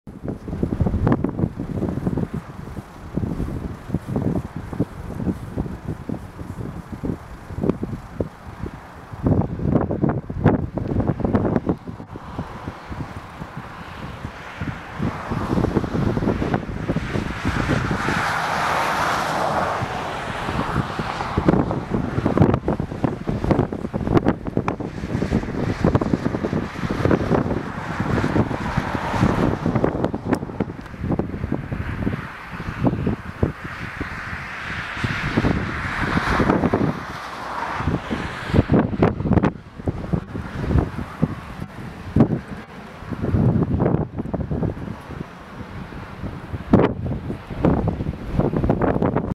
Test Flight / S-70M Blackhawk SP-YVT #95 / The Third S-70M "Romania" / EPML Mielec / 20.02.2023
Test Flight
S-70M Blackhawk SP-YVT #95
The Third S-70M Helicopters
For The Romanian
Ministry Of Internal Affairs
EPML Mielec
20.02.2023